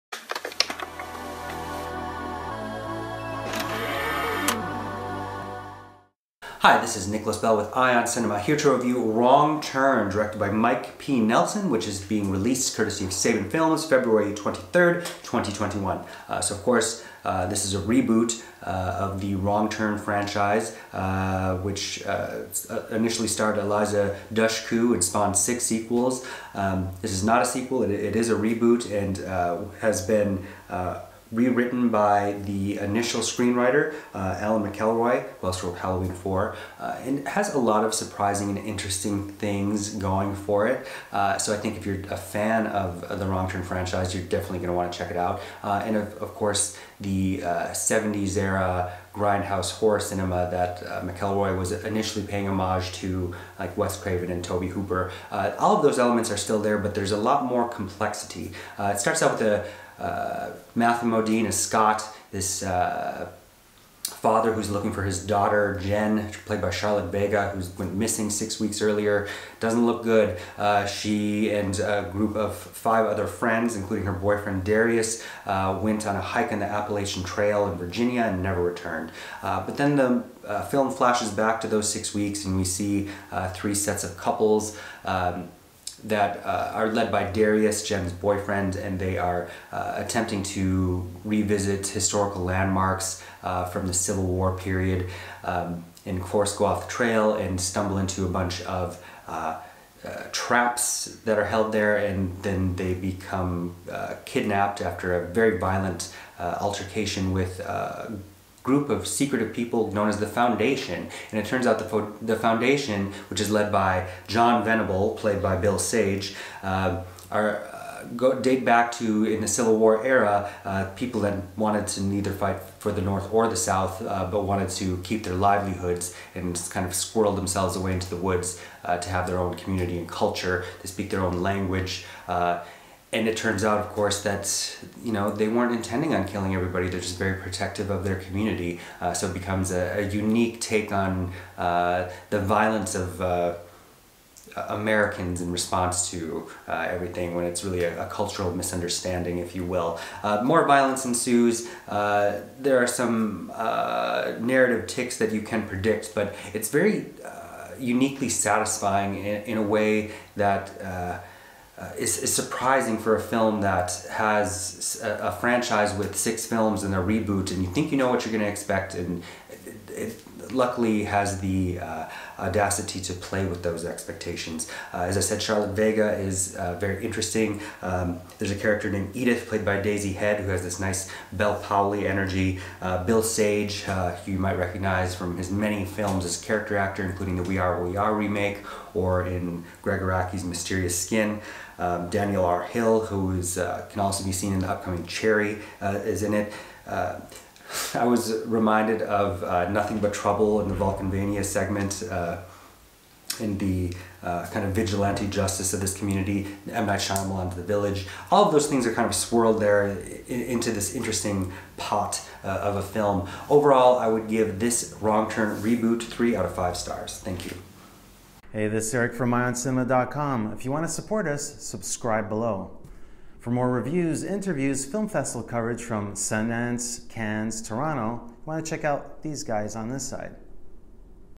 Wrong Turn (2021) - Movie Review
Chief Film Critic Nicholas Bell reviews Mike P. Nelson's Wrong Turn (2021) / IONCINEMA.com.

Film Credits:
Produced by James Harris, Robert Kulzer.
Directed by Mike P. Nelson.
Written by Alan McElroy.
Cinematography: Nick Junkersfeld
Edited by Tom Elkins
Cast: Charlotte Vega, Adain Bradley, Bill Sage, Emma Dumont, Dylan McTee, Daisy Head, Matthew Modine.

Film Rating: ★★★/☆☆☆☆☆